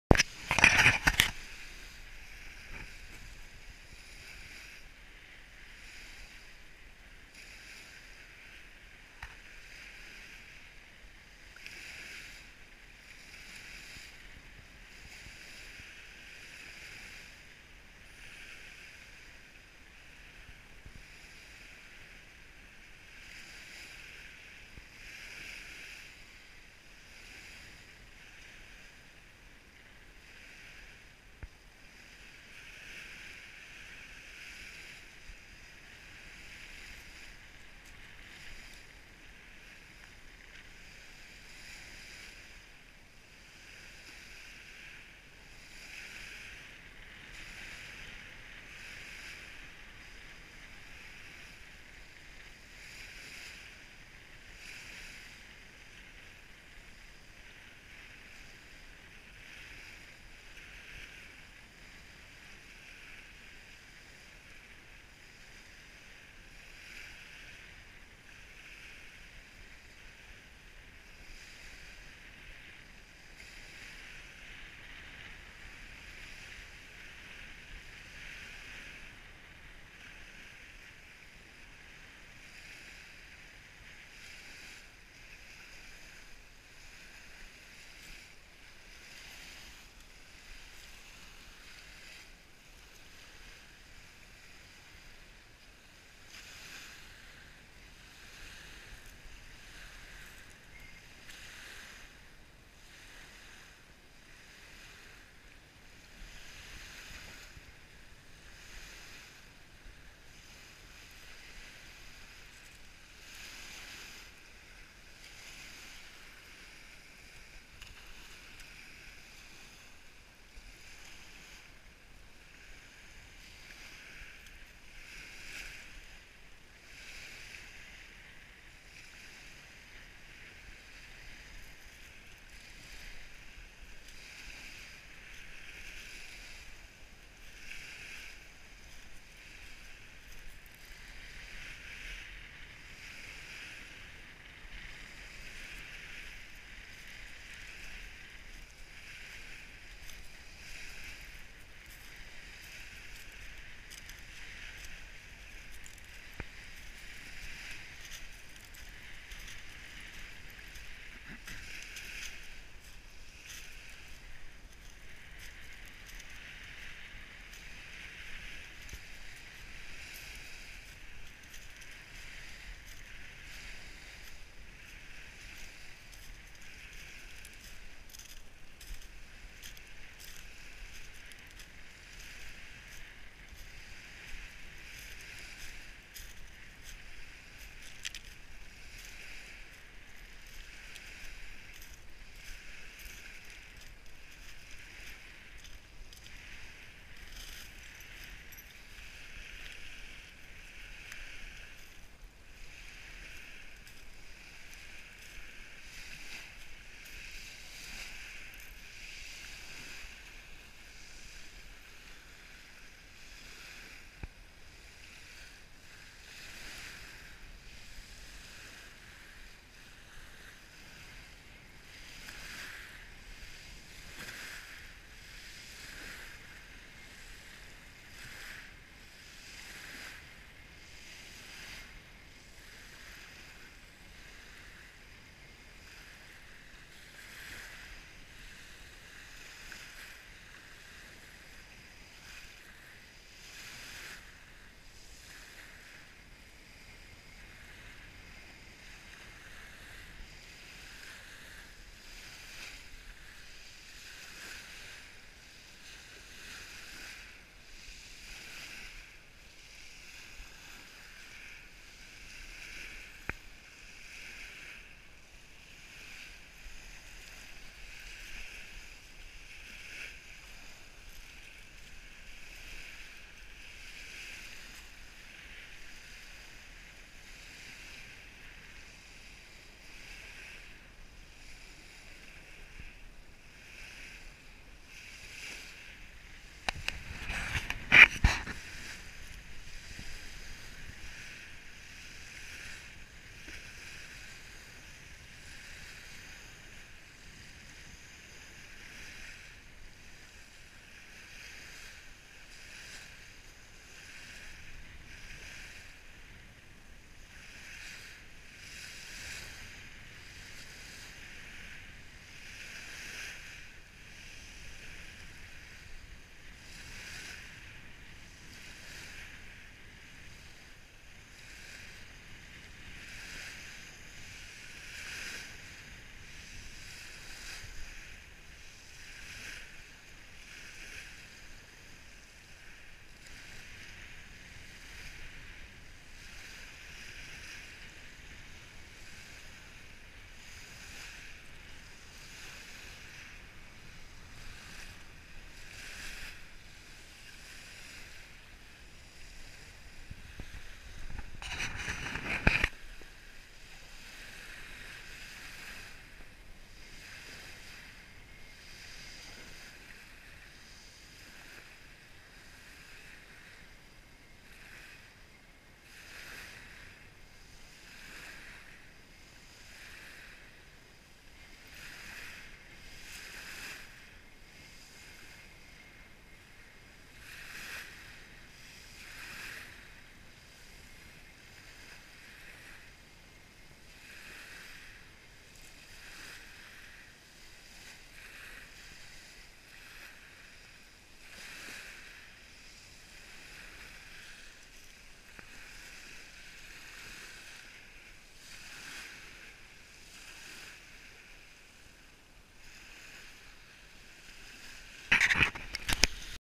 Windsurfen Peljesac, Kroatien, März 2016
Christian Wachter beim Windsurfen auf der Halbinsel Peljesac gegenüber der Insel Korcula